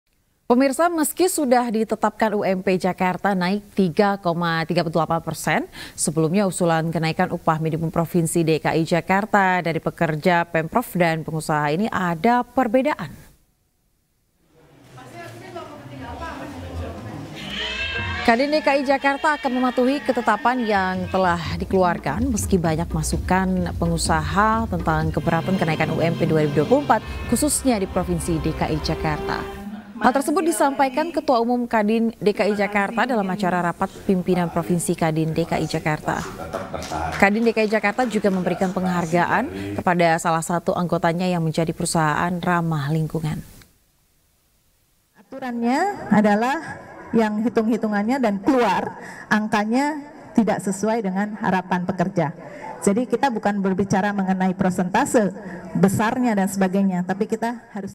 Kenaikan UMP DKI Jakarta, Perbedaan Permintaan Antara Pekerja, Pemprov dan Pengusaha
Pada 21 November 2023, Pemerintah Provinsi DKI Jakarta mengumumkan besaran Upah Minimum Provinsi (UMP) 2024. UMP DKI Jakarta 2024 adalah Rp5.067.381, naik 3,6% dari UMP 2023.